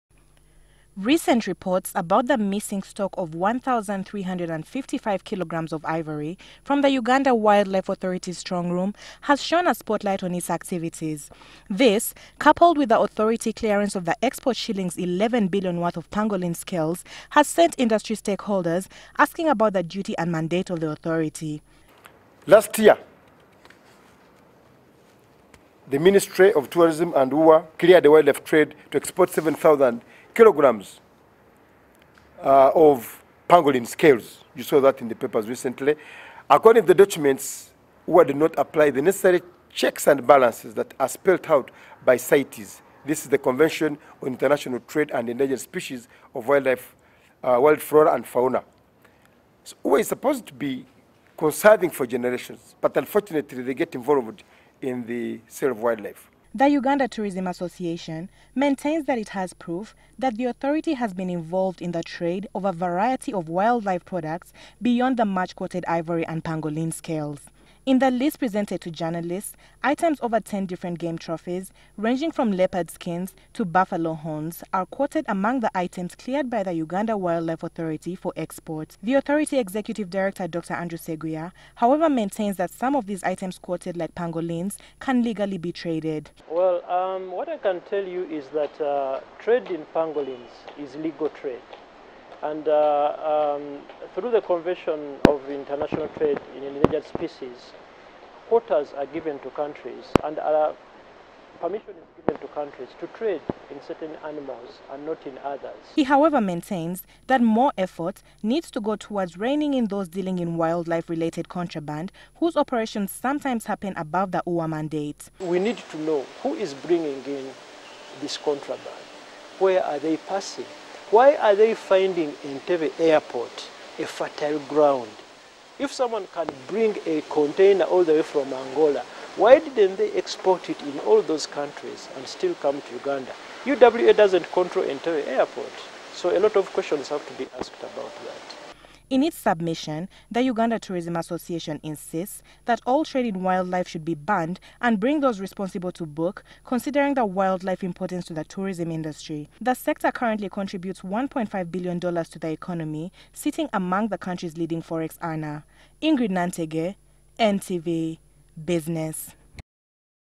Tour Operators caution UWA on game trophies for export
Uganda Wild life Authority should be focused at conserving as opposed to facilitating trade in wild life. Speaking to the press Wednesday the  Uganda Tourism Association noted that it has evidence that the authority has been involved in the collection of game trophies for export including skins of leopards, Cheetahs and pangolin shells.